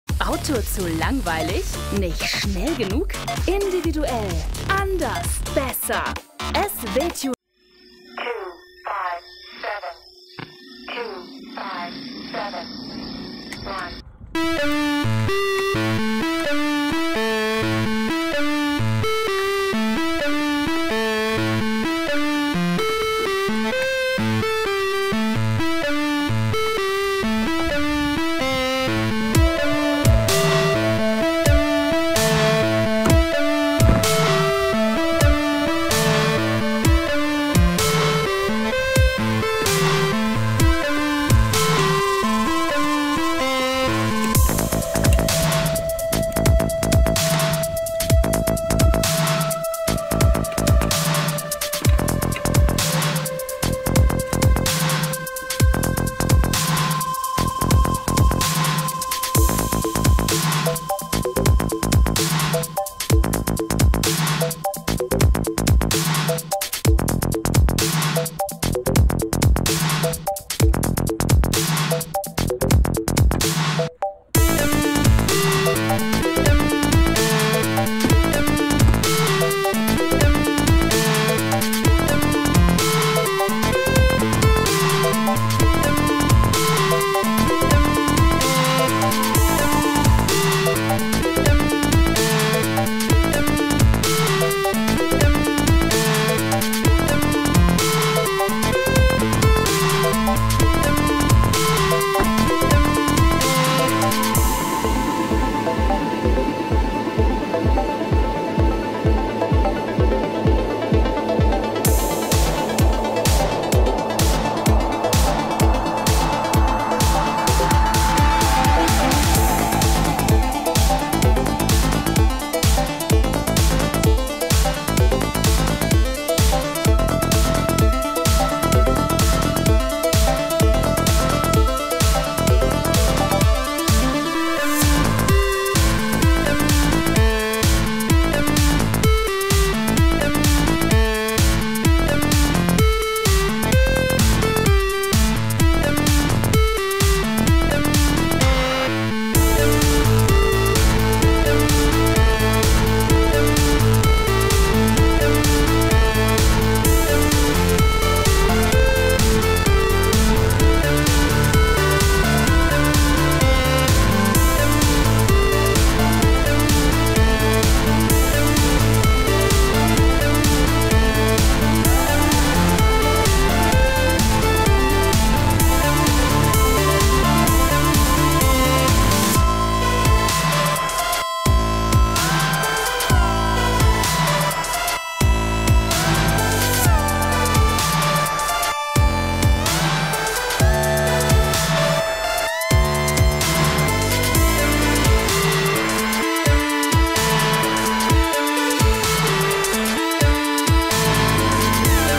SW-Tuning Einbauvideo SW Celi LED Rückleuchten passend für Swift RZ AZ (smoke Version) DIY Tutorial
SW-Tuning präsentiert ein Tutorial "Wie tausche ich bzw. baue ich meine Rückleuchten aus und ersetze Sie passend für Swift Typ RZ AZ Model!"